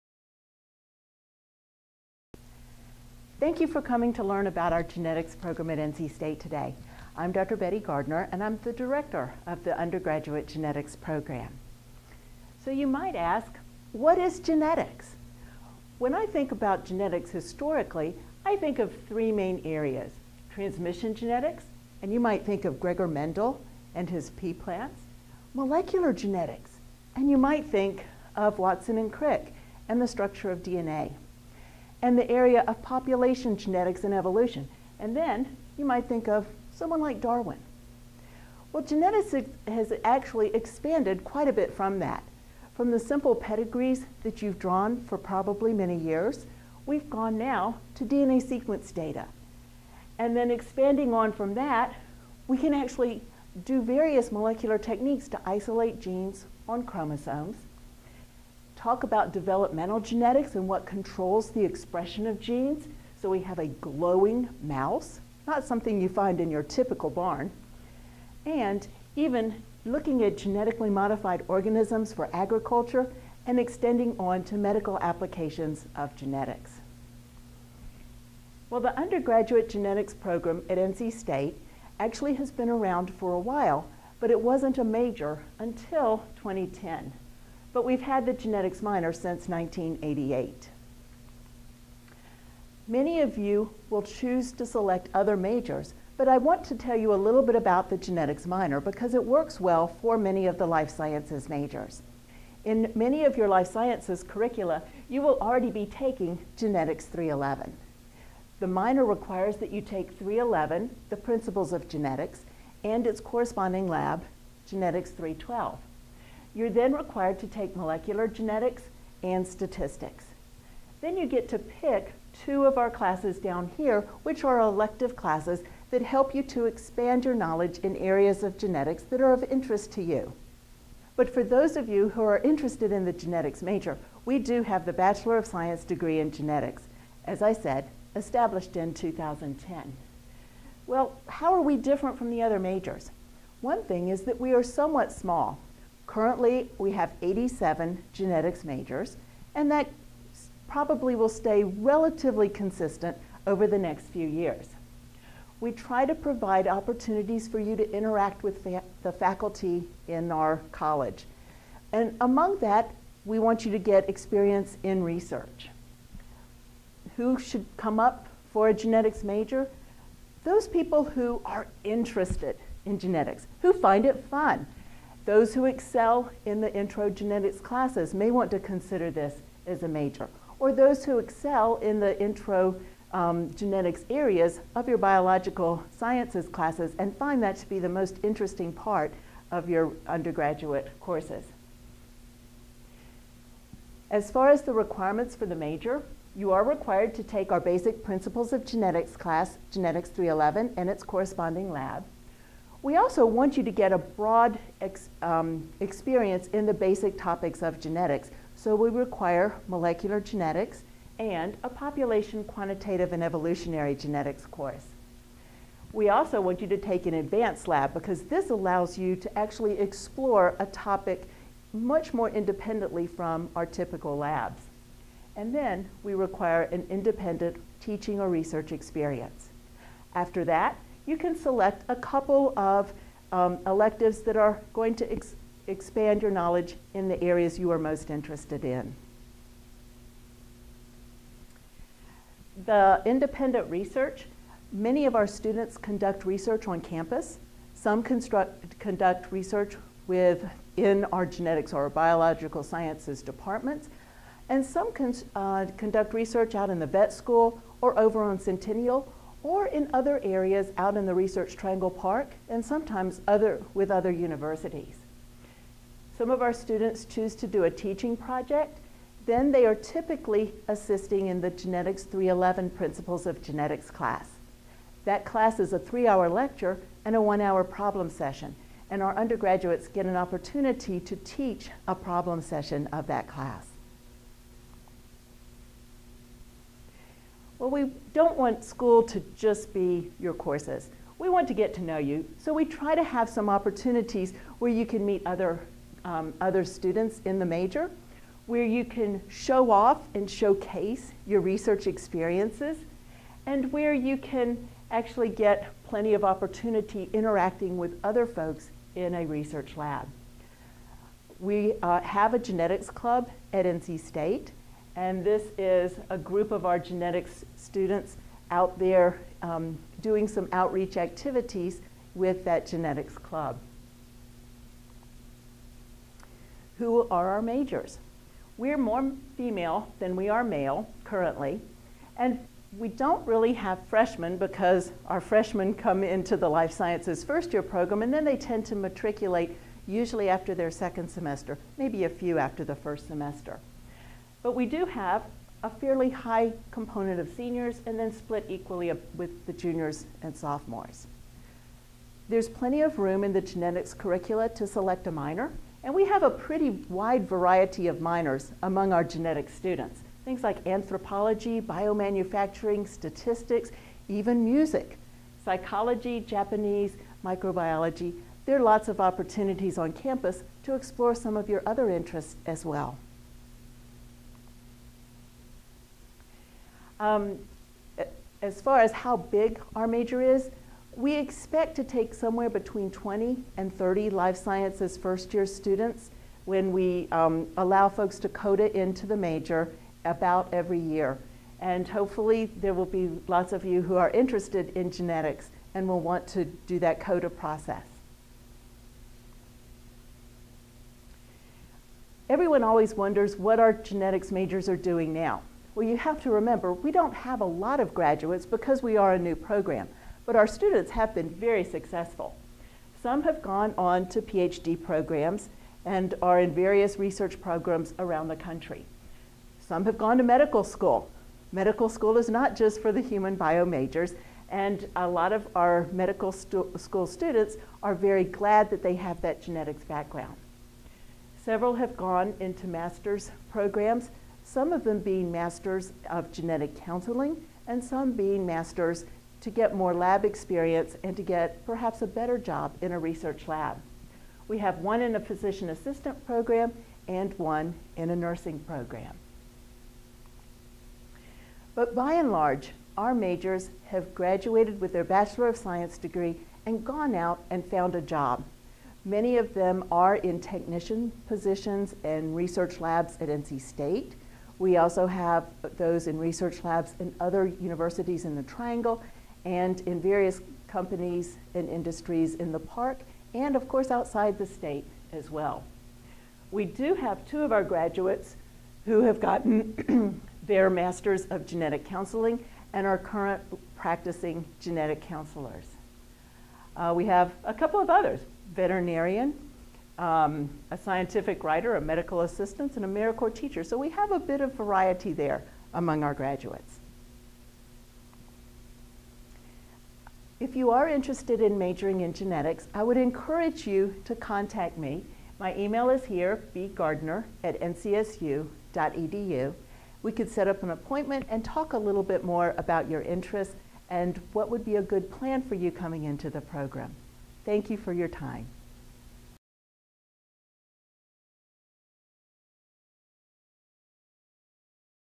Genetics Major - North Carolina State University
Overview of the Genetics Major at NC State University.

Undergraduate Coordinator for the Genetics Program
Dr. Betty Gardner